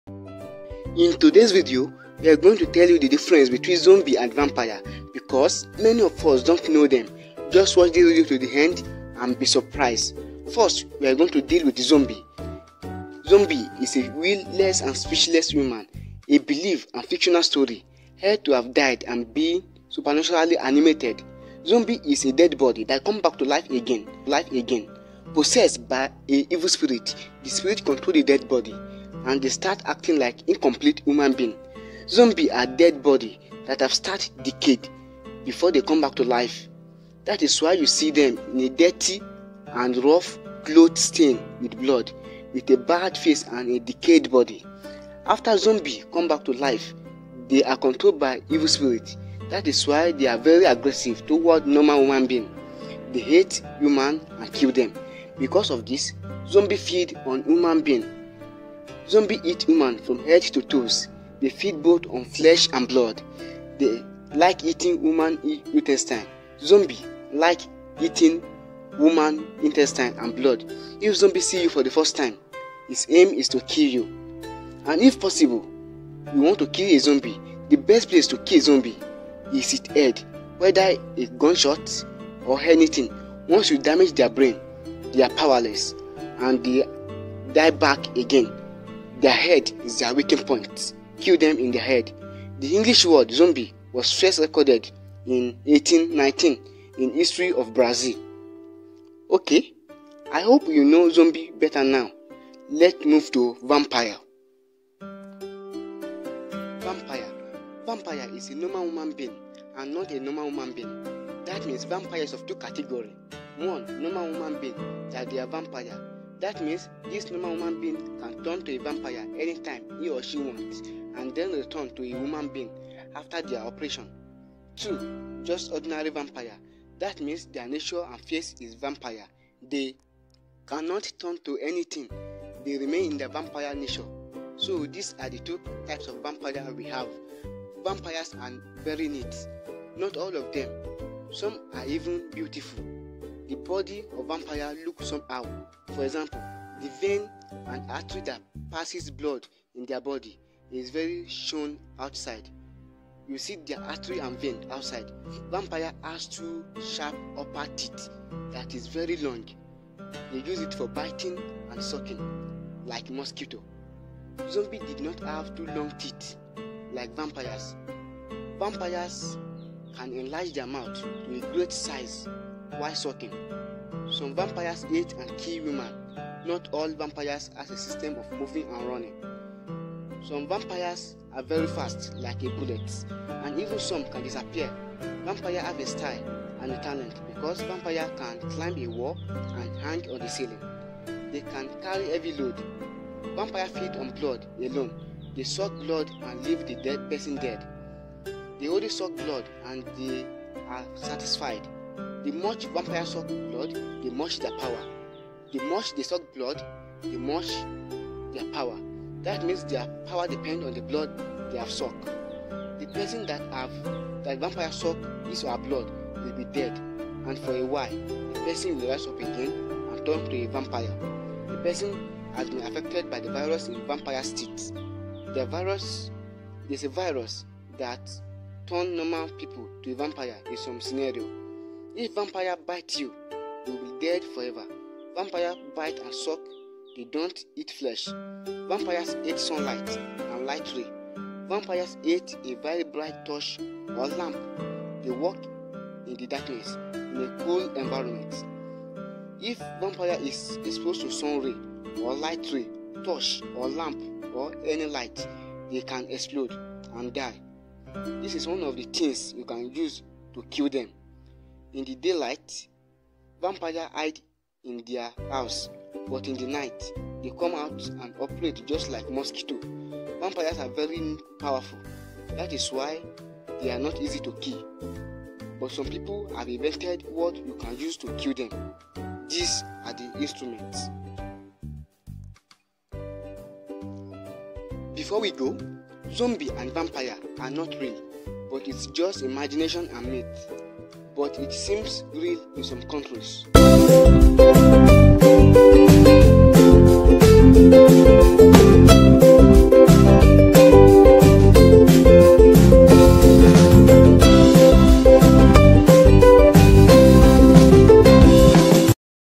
Zombie vs vampire: What Is The Difference.
zombie vs vampire: what is the difference. So in this video, you are going to know difference between vampire and zombie, and their behavior and origination. Try to watch to the end.